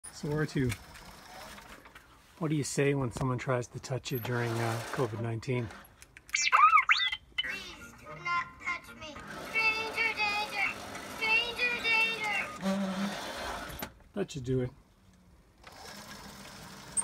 R2D2 - COVID-19
What does R2D2 say when people don't practice social distancing.